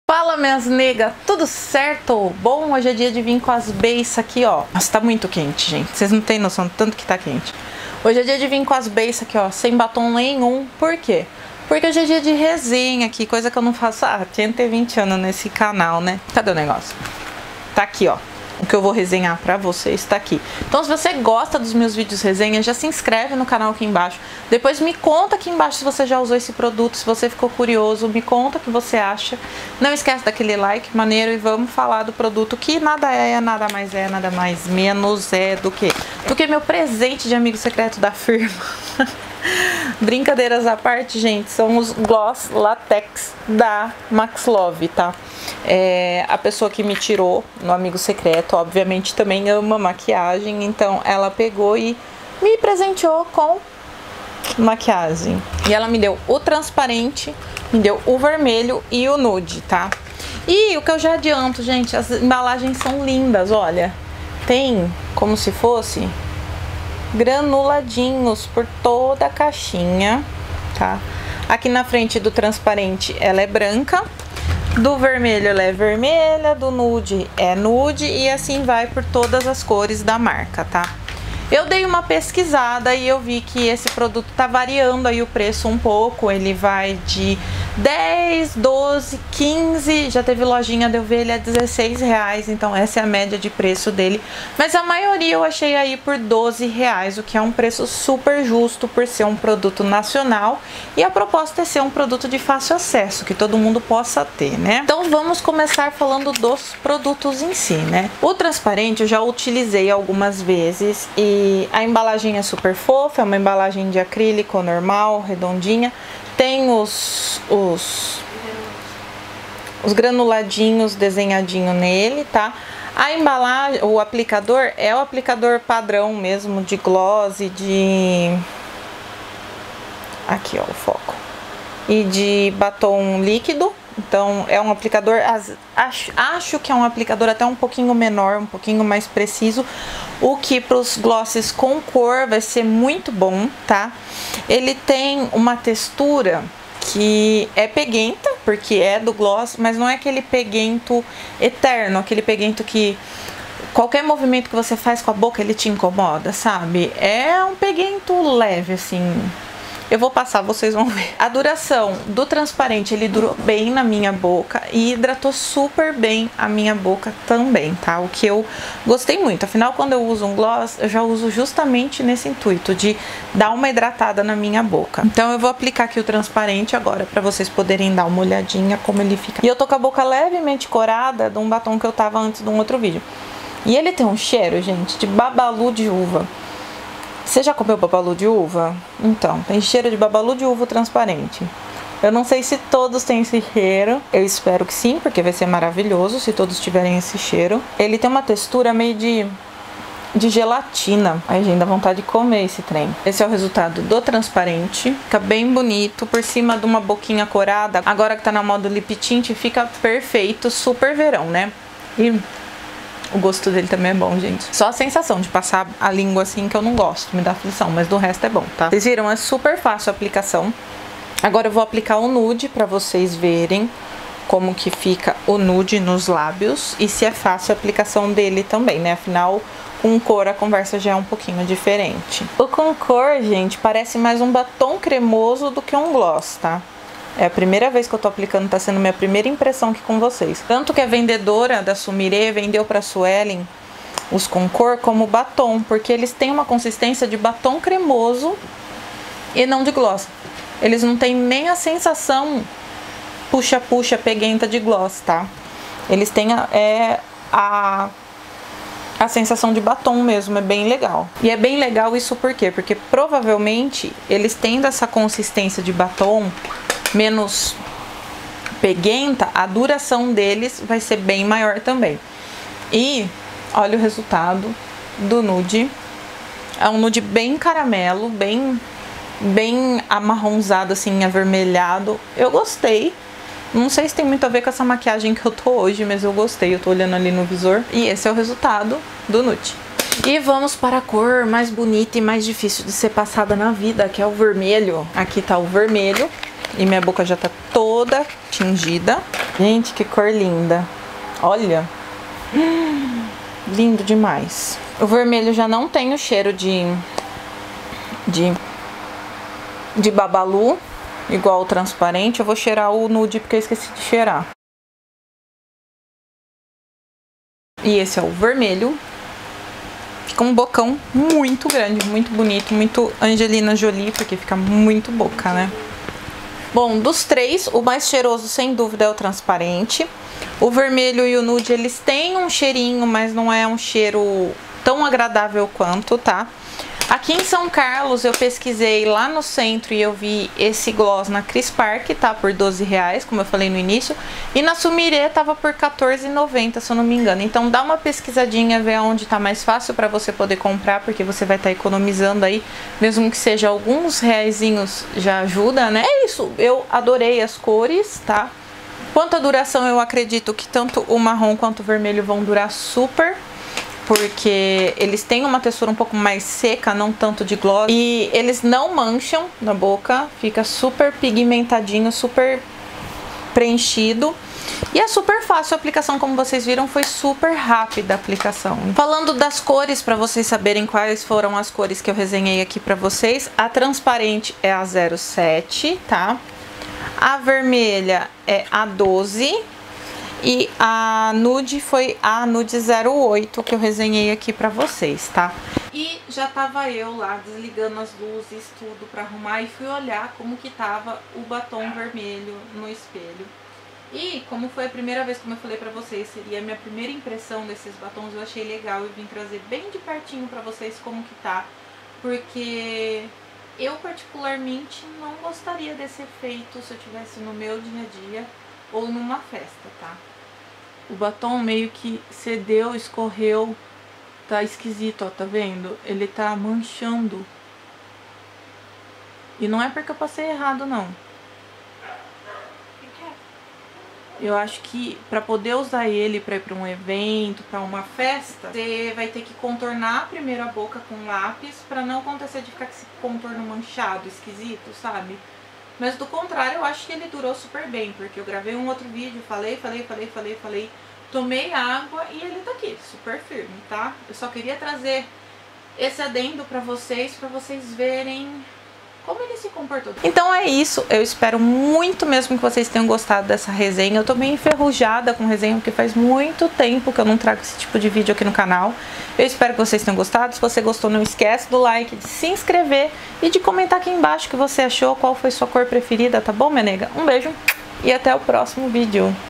Resenha Gloss Latex da Max Love
ABRA AQUI

Oi negas hoje trago uma resenha meio que primeira impressões por aqui sobre um produtinho super BBB os glosses latex da Max love.